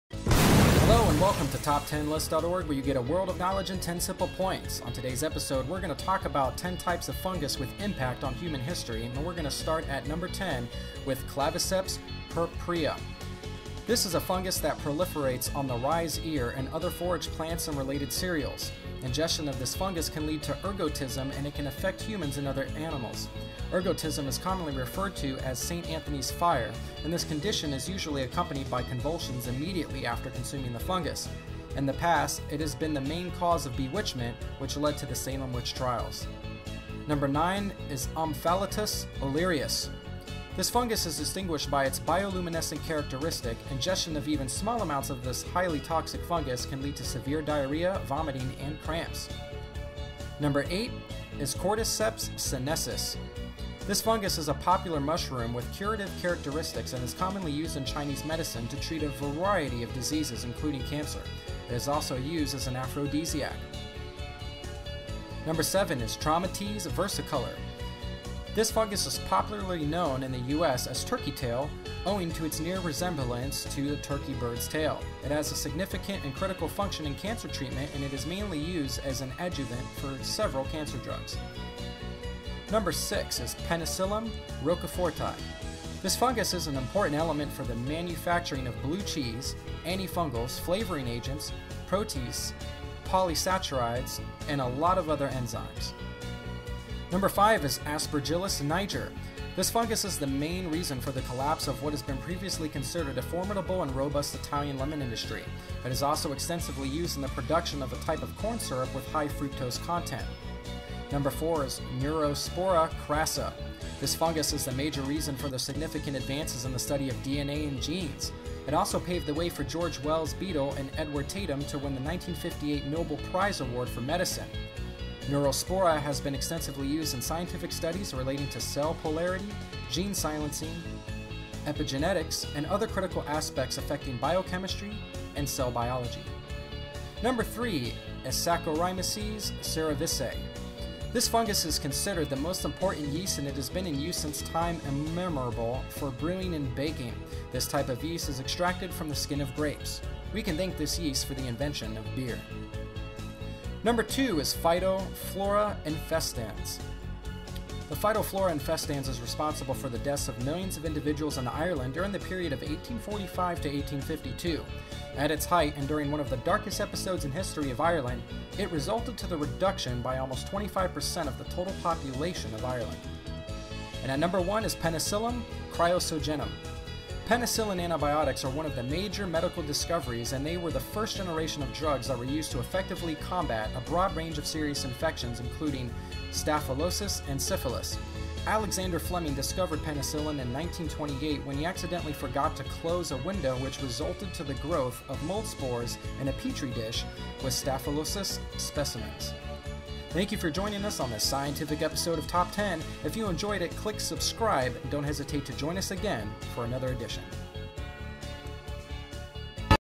Types Of Fungus With Impact On Human History
Funguses have been known to be the major cause of tragedies to human. On the other hand, there are also quite a number of funguses that are instrumental for some of man's greatest discoveries in science. This article presents the list of the top 10 most significant funguses in human history.